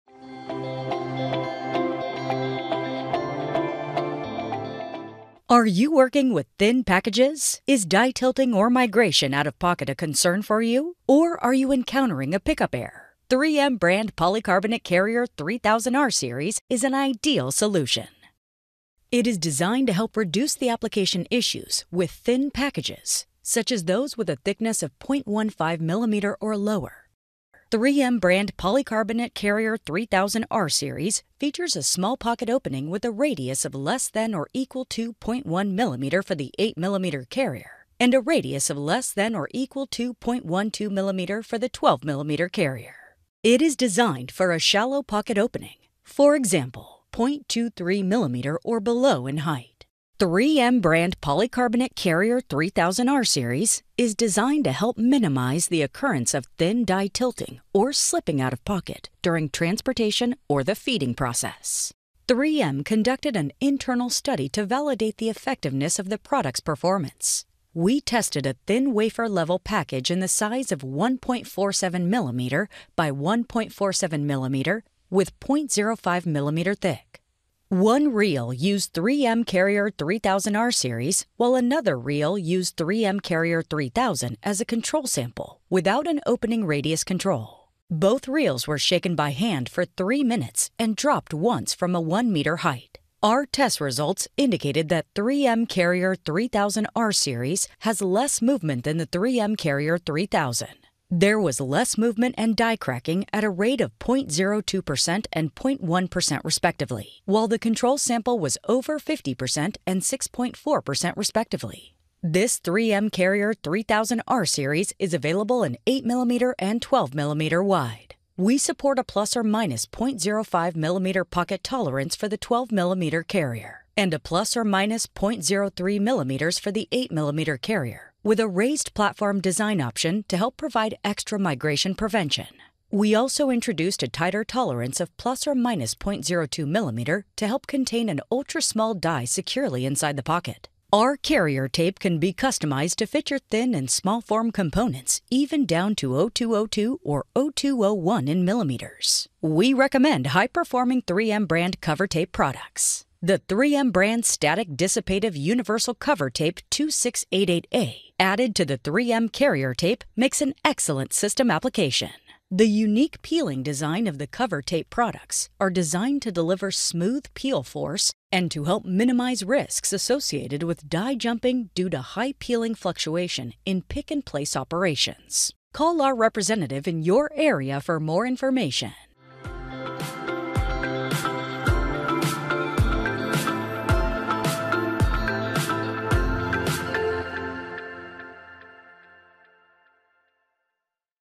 3M™ 導電性エンボスキャリアテープ No. 3000Rシリーズのご紹介
半導体や電子部品の搬送中に薄いチップが飛び出したり、傾いたり、実装エラーが発生することにお困りではありませんか？ 3M™ 導電エンボスキャリアテープ No.3000Rは、コントロールされたポケット開口部を備えており、これらの課題の解決に役立ちます。

3M™ 導電エンボスキャリアテープ No.3000Rは、0.15mm以下の薄いパッケージの課題を軽減するように設計されています。3Mのエンボスキャリアテープのテスト動画をぜひご覧ください。

半導体や電子部品の搬送は、3Mのエンボスキャリアテープにお任せください。